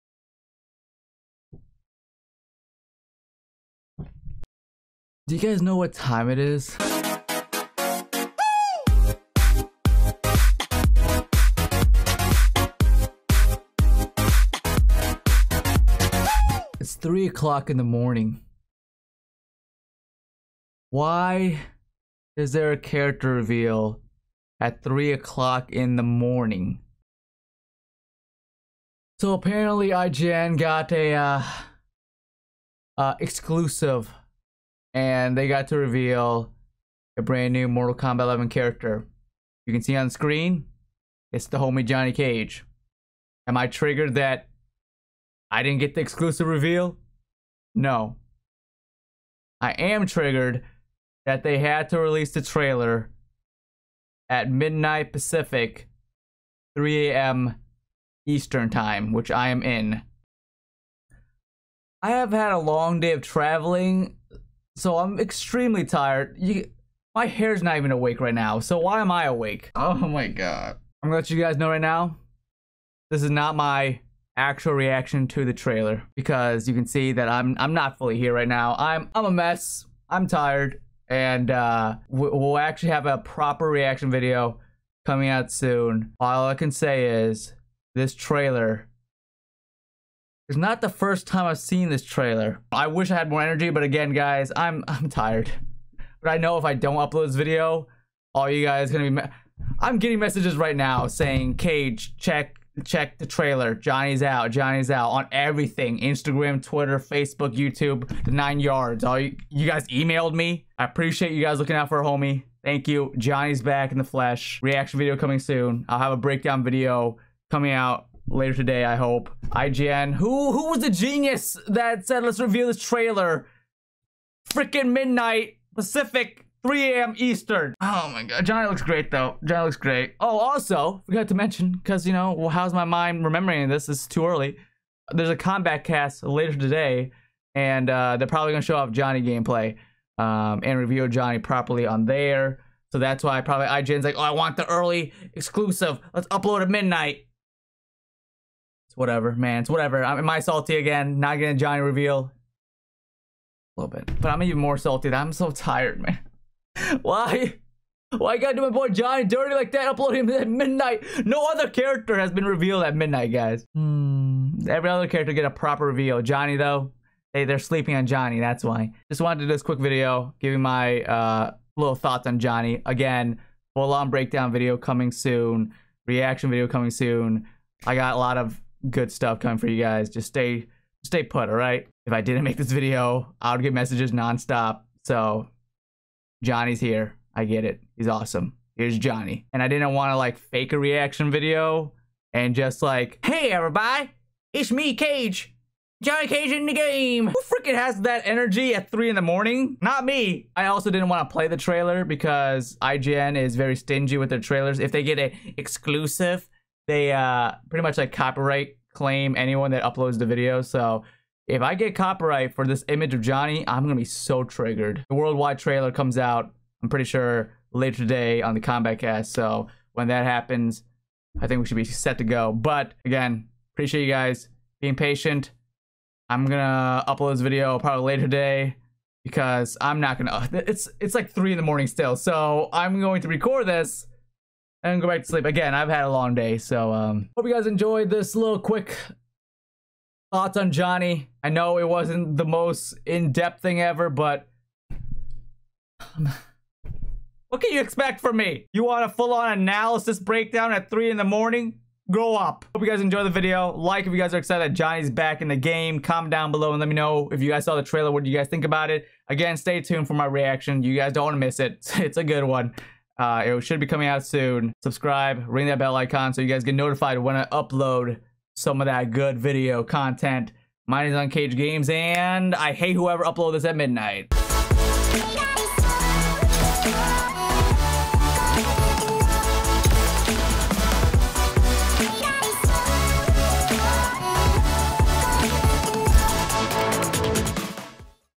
Mortal Kombat 11 - Johnny Cage Got Revealed At 3AM...
Mortal Kombat 11 - Johnny Cage Got Revealed At 3AM... | unCAGEDgamez

So here is my thoughts on Johnny Cage getting revealed for Mortal Kombat 11!... at 3AM!! My reaction to the trailer will be out soon, but in the meantime enjoy sleepless cage talk lol